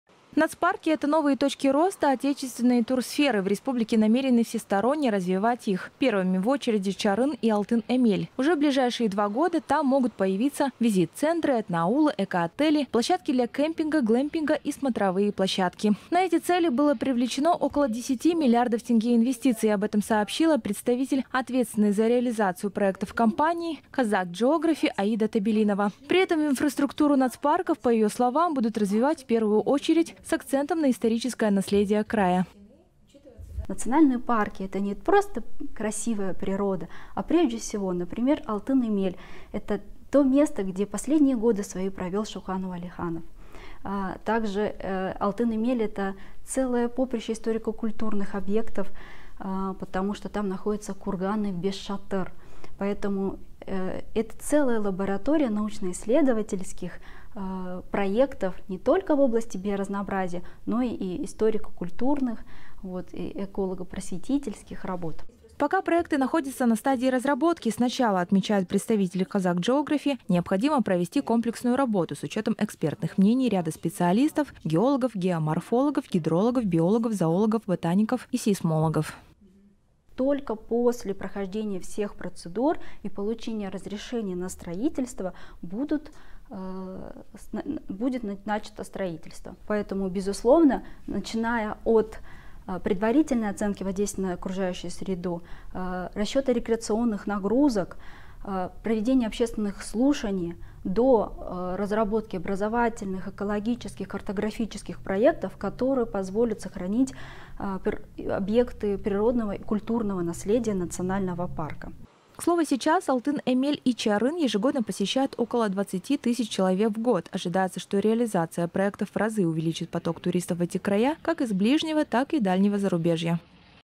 Этноаулы появятся в Национальных парках РК. Новости Qazaq TV
Первыми в очереди - Чарын и Алтын-Эмель.

Нацпарки – это новые точки роста отечественной турсферы. В республике намерены всесторонне развивать их. Первыми в очереди - Чарын и Алтын-Эмель. Уже в ближайшие два года там могут появиться визит-центры, этноаулы, эко-отели, площадки для кемпинга, глемпинга  и смотровые площадки. На эти цели было привлечено  около 10 млрд тенге инвестиций. Об этом сообщила Представитель ответственной за реализацию проектов компании «Qazaq geography» Аида Табелинова. При этом инфраструктуру нацпарков, по ее словам, будут развивать, в первую очередь,  с акцентом на историческое наследие края.

Аида Табелинова,  кандидат географических наук,   главный менеджер «Qazaq Geography»:

- Национальные парки  - это не просто красивая природа. К примеру, Алтын Эмель  -это  то место, где последние годы  своею жизнь провел Шокан Уалиханов, это целое поприще историко-культурных объектов , потому что там находятся курганы Бесшатыр. Поэтому это целая лаборатория научно-исследовательских проектов не только в области биоразнообразия, но и историко-культурных,  эколого-просветительских работ.

Пока проекты находятся  на стадии разработки. Сначала, отмечают представители «Qazaq geography», необходимо провести комплексную работу с учетом экспертных мнений ряда специалистов  -  геологов, геоморфологов, гидрологов, биологов, зоологов, ботаников и сейсмологов.

- Только после прохождения всех процедур и получения разрешения будет начато строительство.  Поэтому, безусловно, мы начнем от  предварительной оценки воздействия на окружающую среду, расчета рекреационных нагрузок, проведения общественных слушаний до разработки образовательных, экологических, картографических проектов, которые позволят сохранить объекты природного и культурного наследия национального парка.

К слову, сейчас Алтын Эмель и Чарын ежегодно посещают около 20 тысяч человек  в год.  Ожидается, что реализация проектов в разы увеличит поток туристов  в эти края,  как из ближнего, так и дальнего зарубежья.